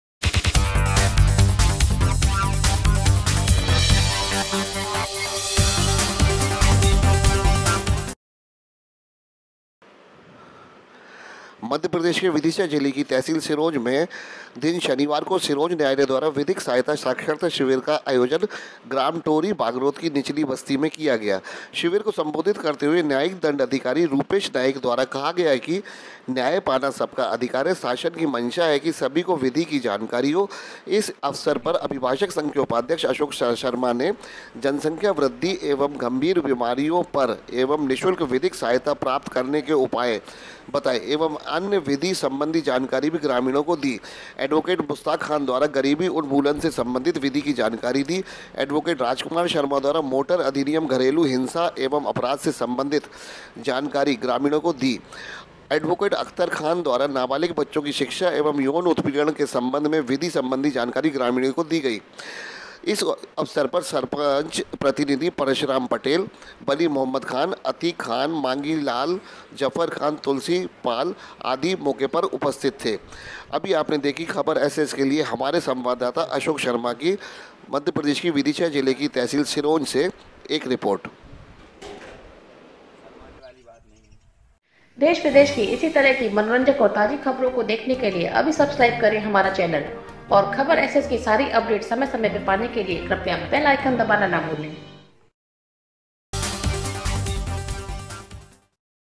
Sironj me Vidhik saksharta shivir ka ayojan hua.
Sironj se khabar SS ki report.